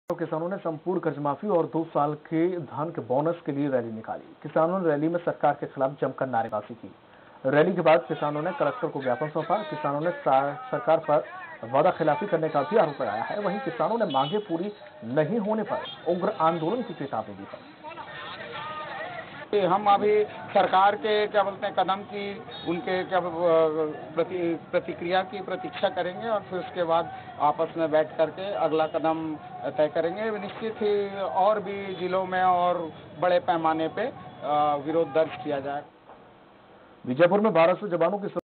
Rajnandgaon News: भूपेश सरकार के खिलाफ किसानों ने किया प्रदर्शन
Bhupesh Baghel CMO Chhattisgarh Ravindra Choubey